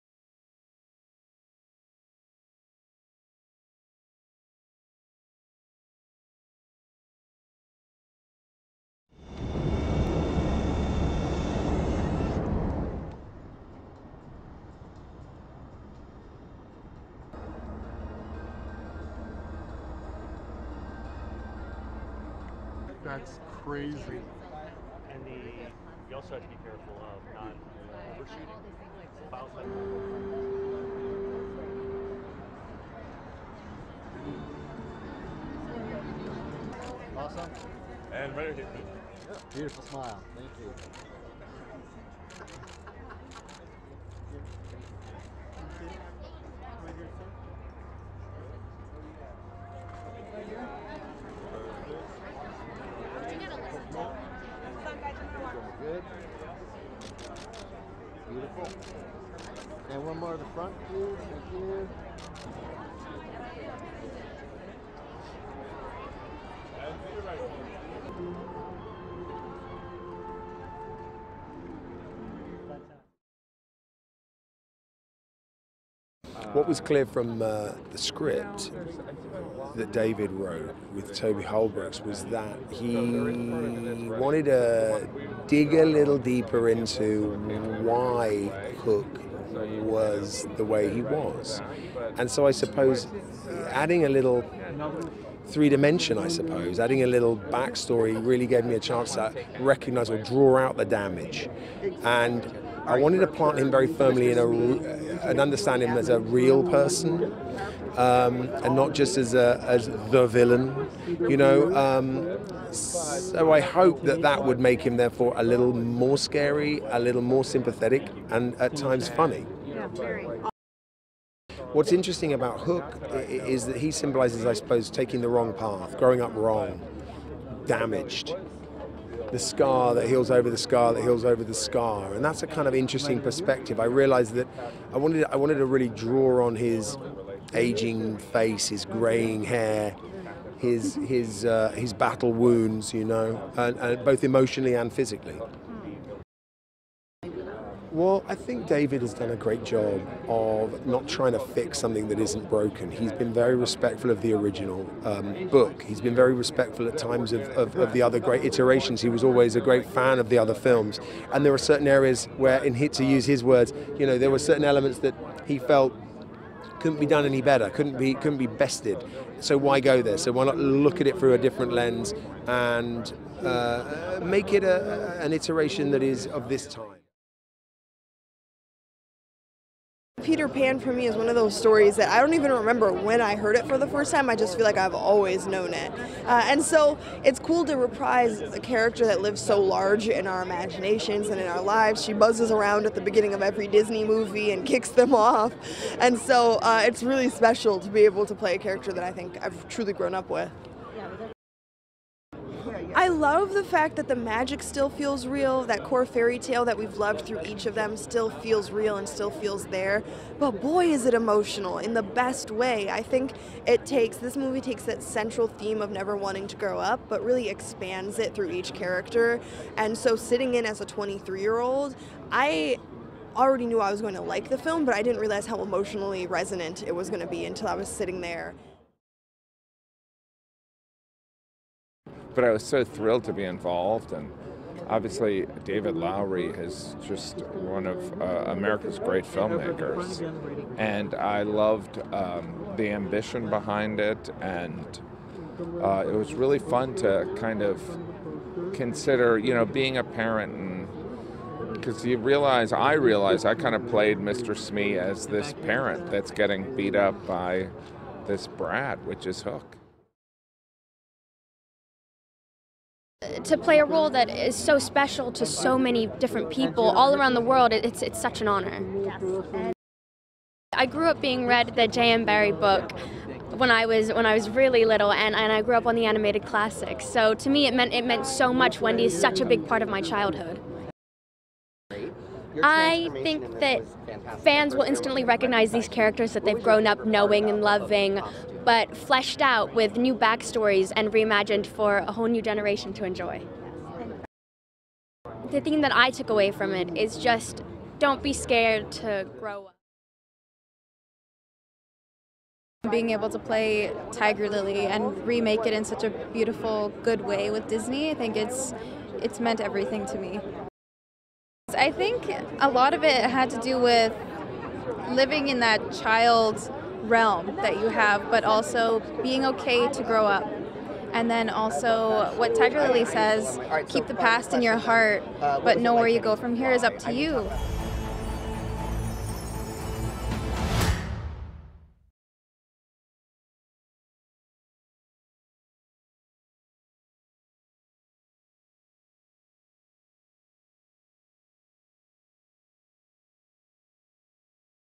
"Peter Pan & Wendy" New York Special Screening Red Carpet
"Peter Pan & Wendy" New York Special Screening Red Carpet interviews\
     Live-action adaptation of J.M. Barrie's classic tale of a boy who wouldn't grow up and recruits three young siblings in London to join him on a magical adventure to the enchanted Neverland island.
      Cast: Alexander Molony, Ever Anderson, Joshua Pickering, Jacobi Jupe, Yara Shahidi, Jude Law,  Jim Gaffigan, Noah Matthews Matofsky, Sebastian Billingsley-Rodriguez, Kelsey Yates,
Florence Bensberg
     Directed by: David Lowery